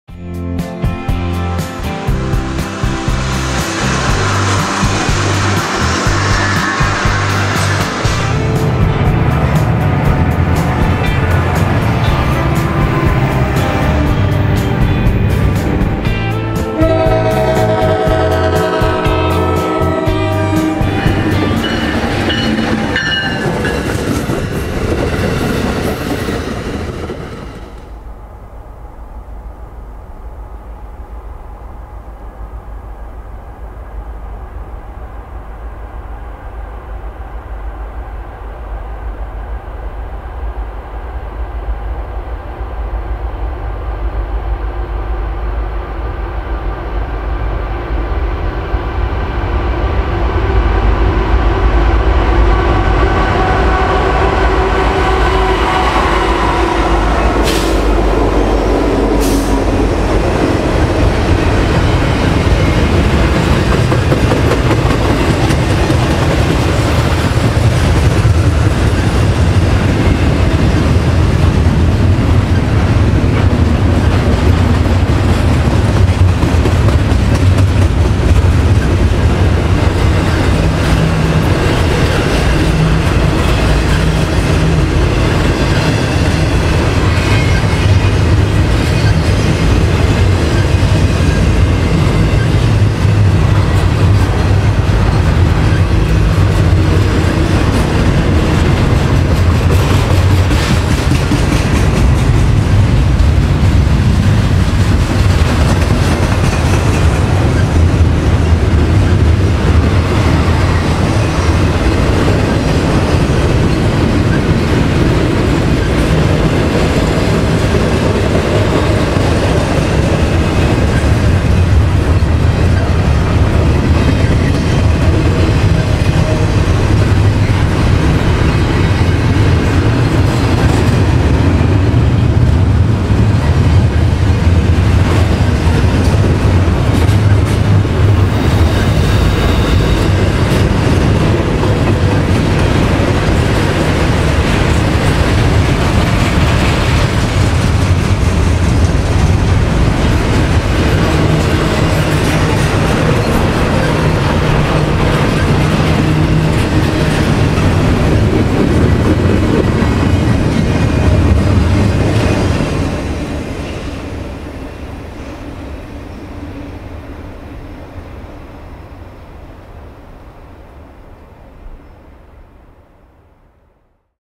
FXE 4066 Trailing On NS 23Z!! 2/16/22
I was in the Easton, PA area when a buddy of mine said that NS 23Z had a Ferromex engine trailing! I headed over to the Phillipsburg Railroad Historians Museum in Phillipsburg, NJ to wait for it on the NS Lehigh Line!! It wasn’t long until they got the clear signal and came on by!! It’s not often to see foreign power this way on NS much anymore.. Especially Ferromex! It was definitely cool to see one for the first time as well! I hope y’all enjoy and as always thanks for the love and support!!